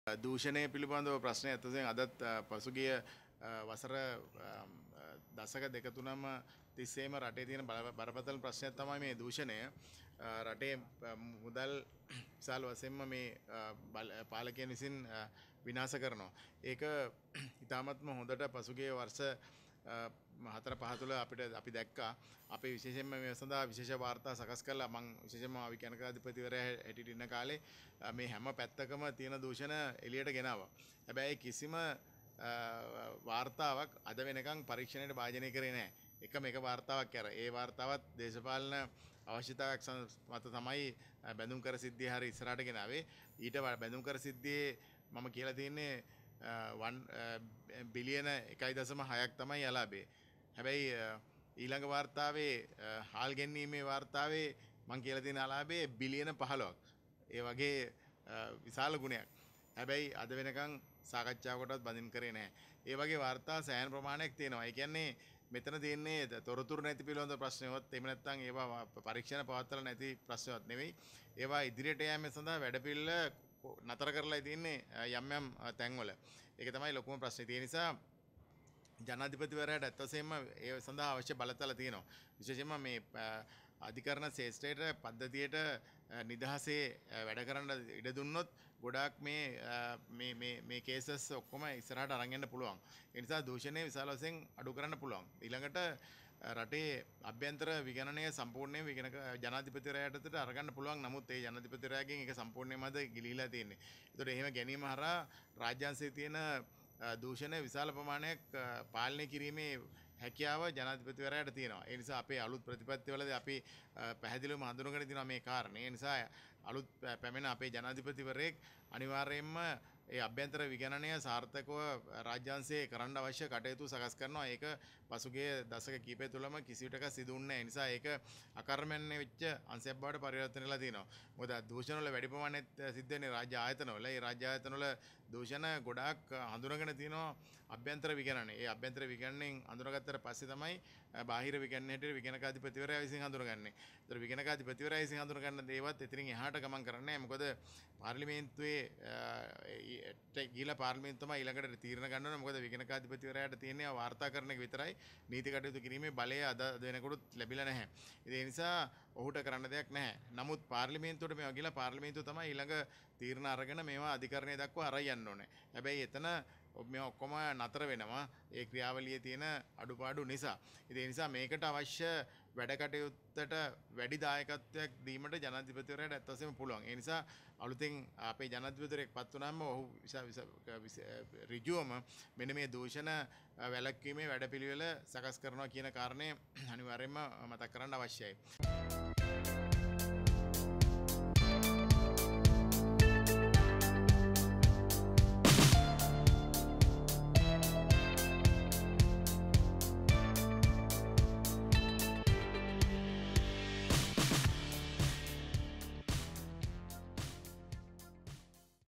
Gamini Wijesinghe speech - බැදුම්කරයට වඩා විශාල සහල් ගෙන්වීමේ වංචාව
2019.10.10
ජාතික ජනතා ව්‍යාපාරය - මහනුවරදී පැවති මාධ්‍ය හමුව
National Peoples Movement